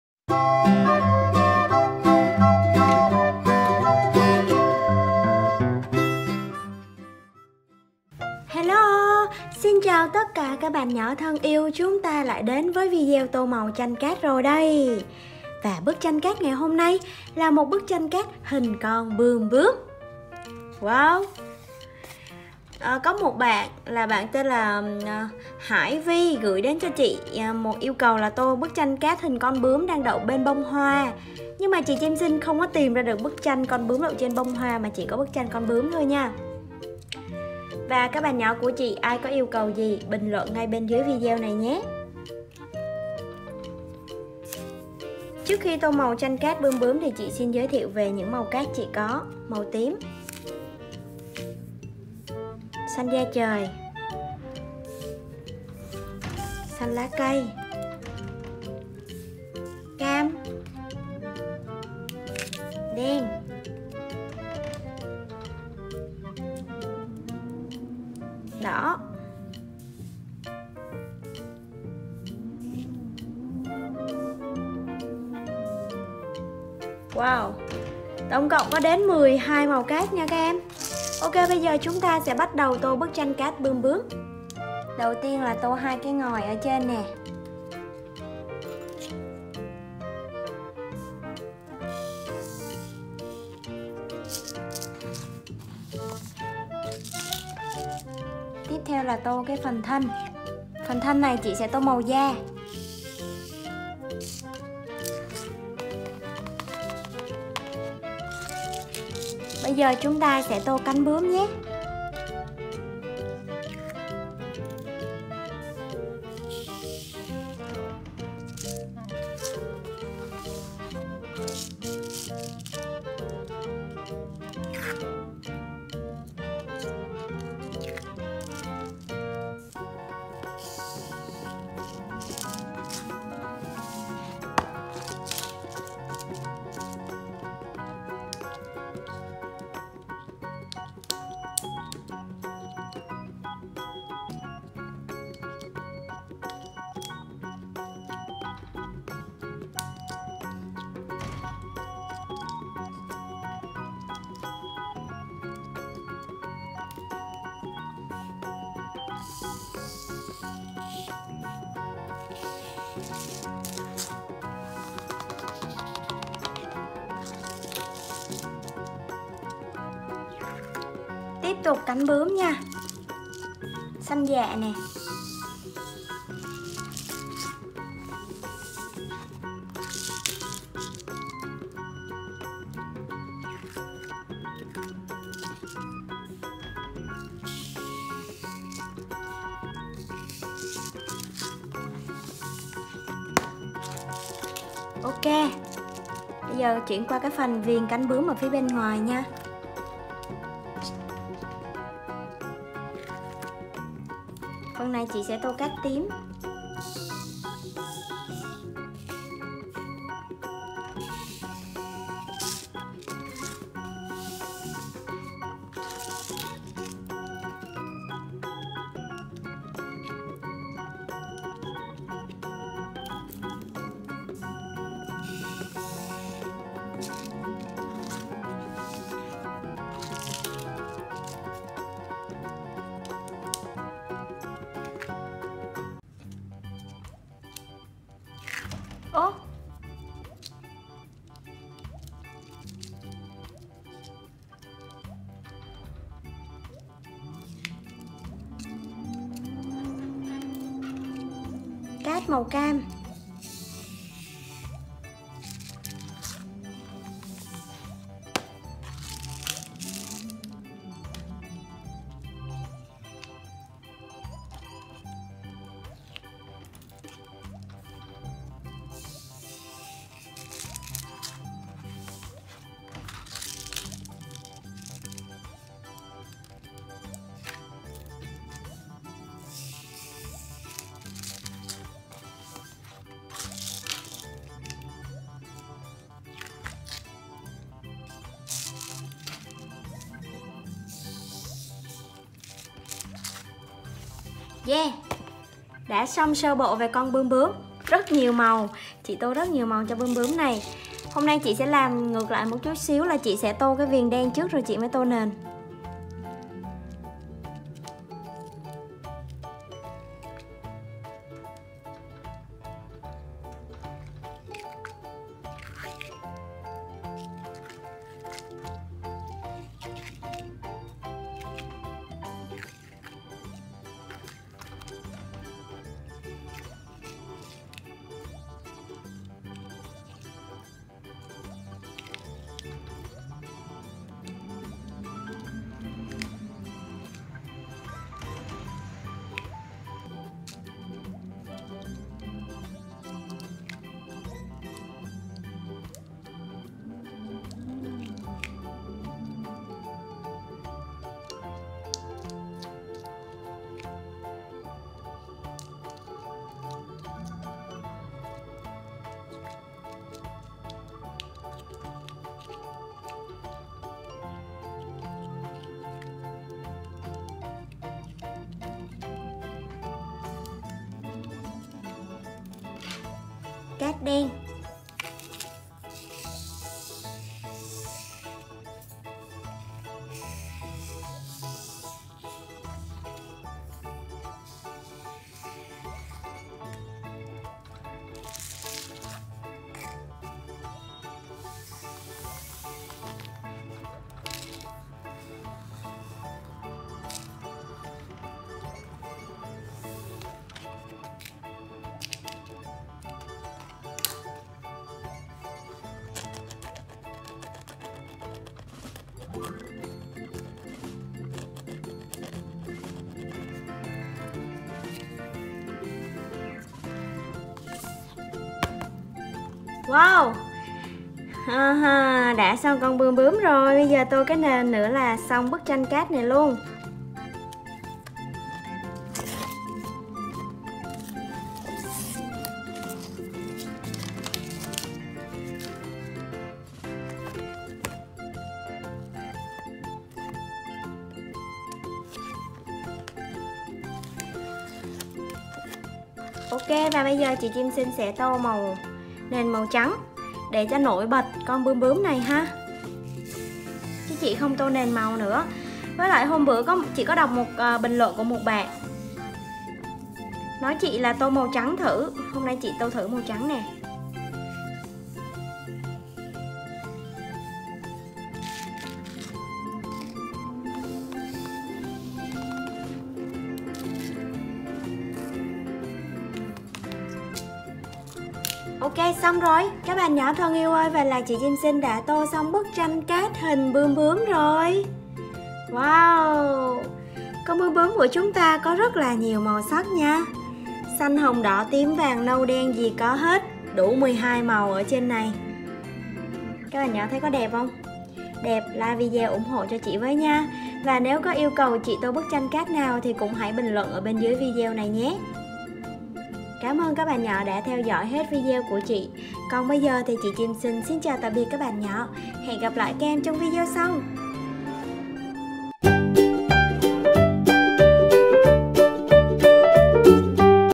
TÔ MÀU TRANH CÁT BƯƠM BƯỚM XINH - Colored sand painting butterfly (Chim Xinh)
Chị Chim Xinh chơi TÔ MÀU TRANH CÁT BƯƠM BƯỚM XINH cho các em xem, trong video này sử dụng 12 màu cát khác nhau để tô màu